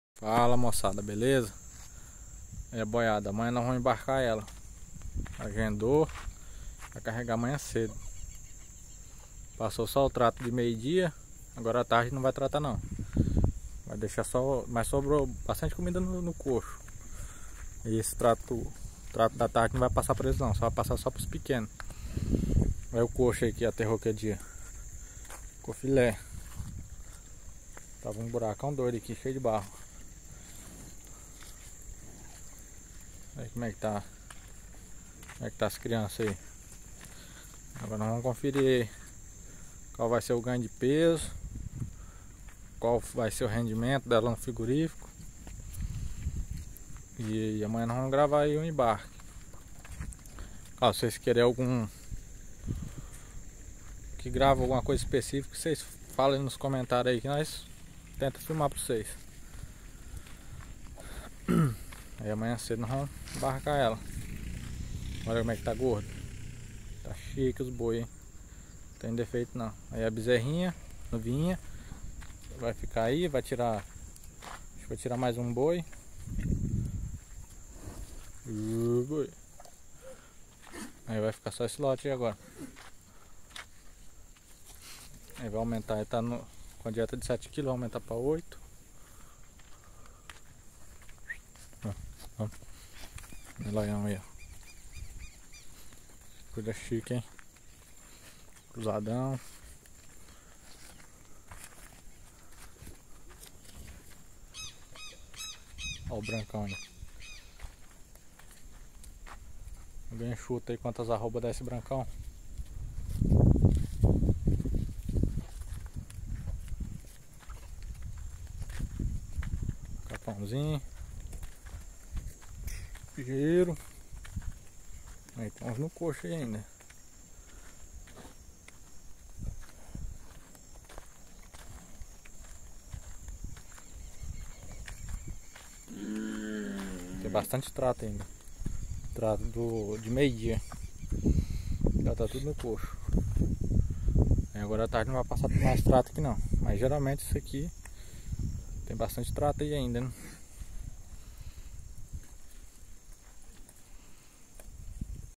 Amanhã tem embarque dos bois do confinamento
fala moçada beleza último dia do confinamento com silagem de milho.
amanhã vai ter embarque da Boiada

@pecuária boi gordo

Meu canal tem intuito de compartilha
Experiência na pecuária